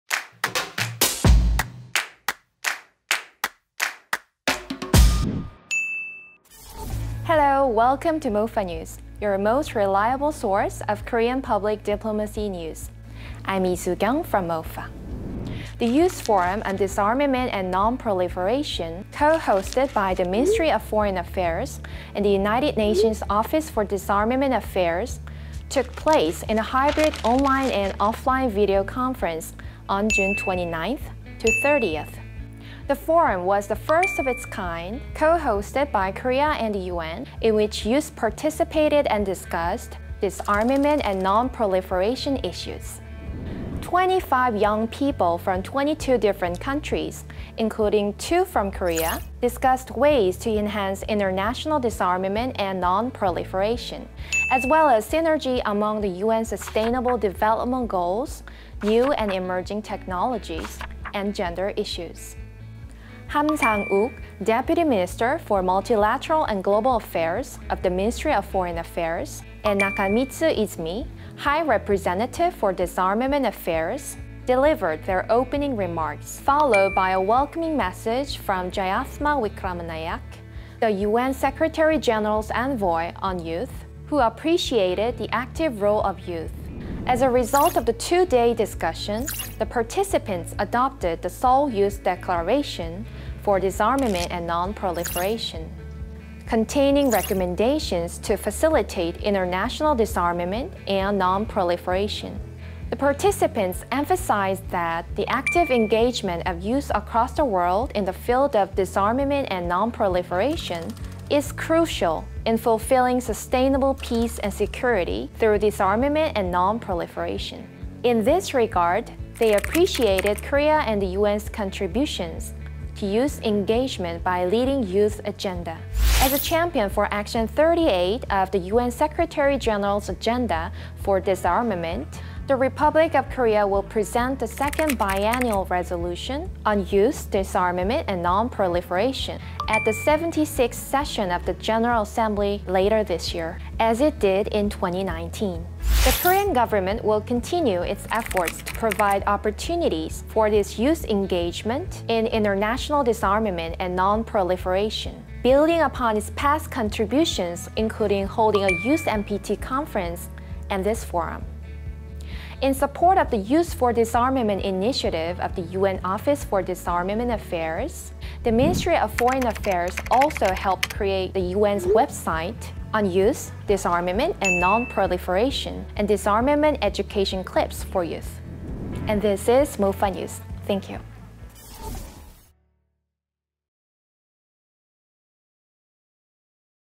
[MOFA NEWS] Youth Forum on Disarmament and Non-Proliferation Took Place
The Youth Forum on Disarmament and Non-Proliferation co-hosted by the Ministry of Foreign Affairs and the United Nations Office for Disarmament Affairs took place on June 29 to 30. The Korean government will continue its efforts to provide opportunities for this youth engagement in international disarmament and non-proliferation.

외교부는 유엔 군축실과 공동 주관으로 ｢한-유엔 청년과 군축·비확산 포럼｣을 6.29-30간 개최했습니다. 우리 정부는 국내외 청년들의 군축·비확산 논의 참여를 장려하기 위한 다양한 사업들을 계속 추진해 나갈 예정입니다.